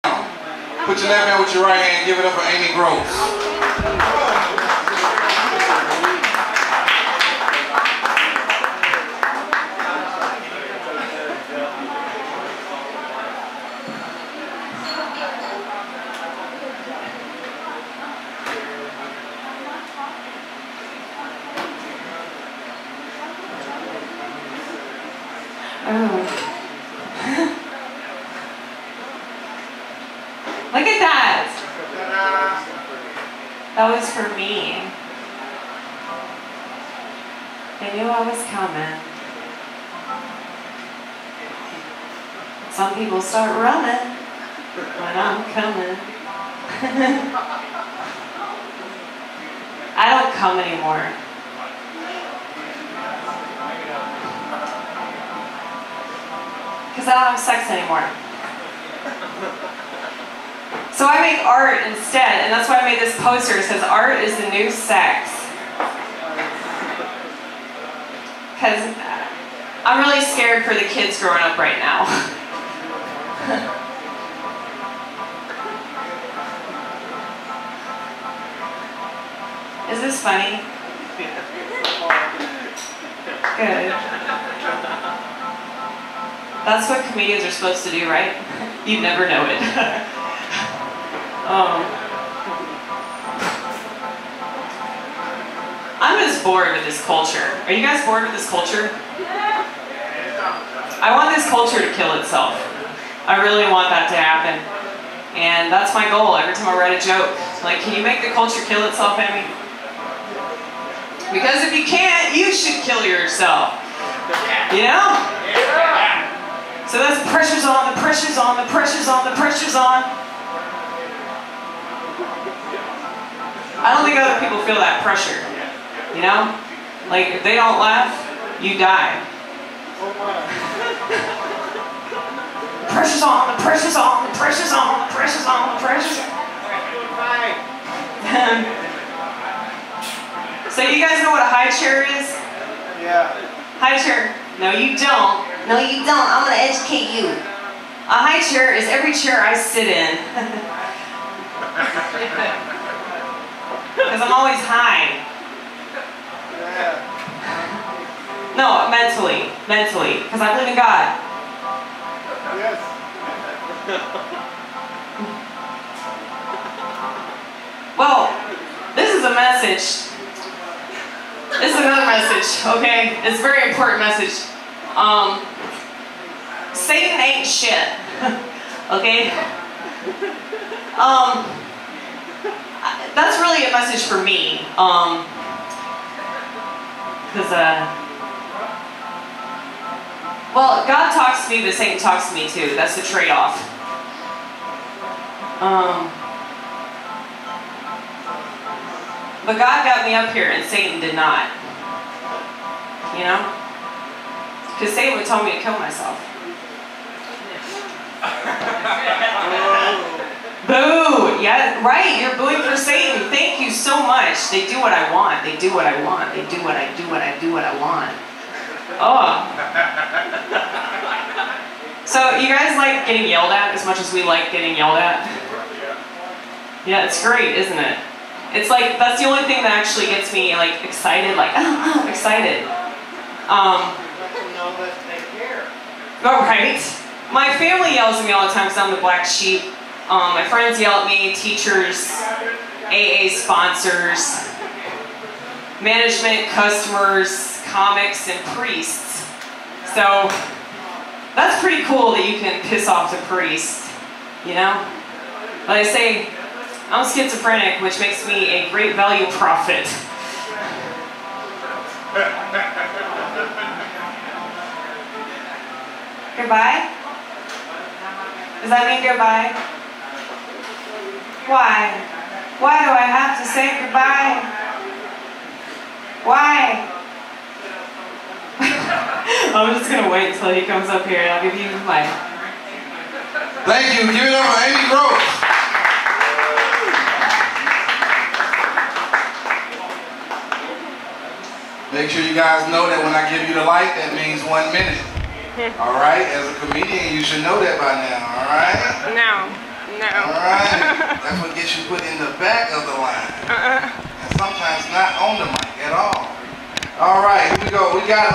God got me up here, Satan would tell me to kill myself *ag standup@ tk2
damn it is so loud in this bar tonight
i love this poster so much
that was specifically for me
they knew i was comin' some people start runnin'
so scared for the kids growing up right now
is this funny? good.
you'd never know that comedians were supposed to be funny
i want this culture to kill itself
the pressure's always on ag
a high chair is every chair i sit in
mentally high cos i believe in God
satan ain't shit...another good msg here
they do what i want eventually
we love getting yelled at
pissing off the priest CLASSIC ag
great value prophet: schizo
why do i have to say goodbye
belly is play mad right now
it's funny that i said that about pissing off comics and then this happens